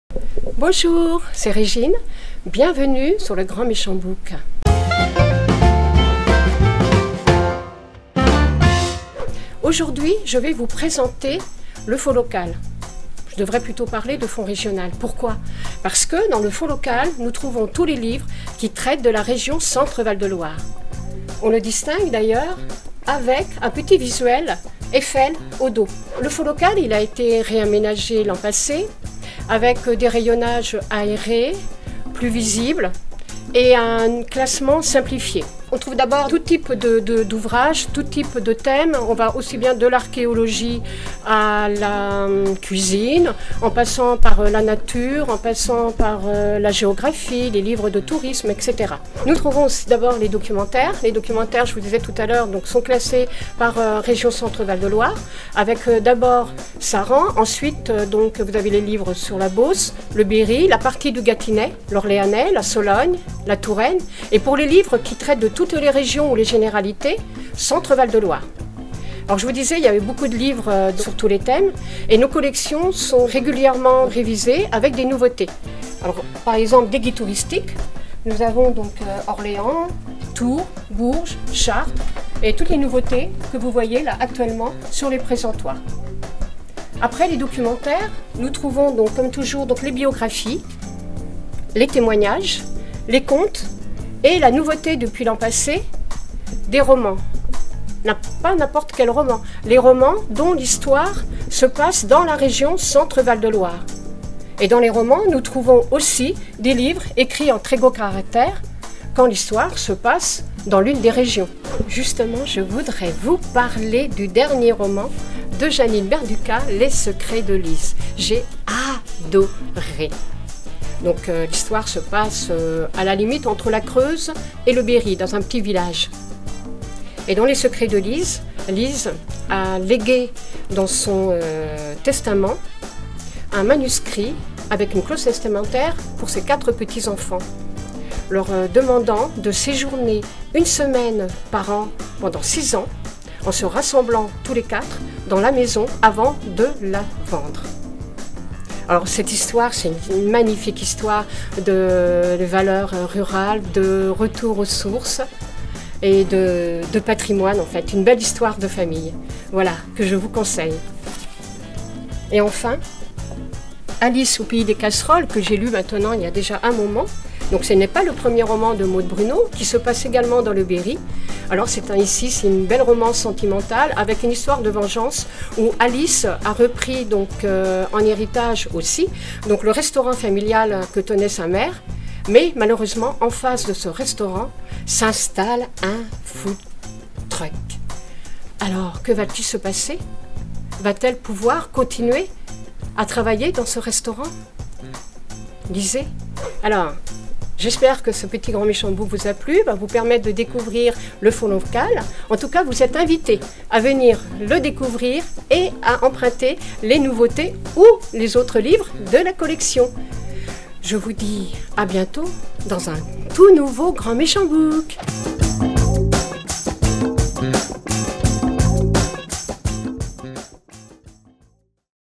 Découvrir les livres locaux - Grand Méchant Book #51
En ce mois de mai, Régine nous fait découvrir le fonds local de la médiathèque, un espace dédié aux livres régionaux. Elle en profite pour nous présenter deux ouvrages.

Retrouvez ces livres à la médiathèque !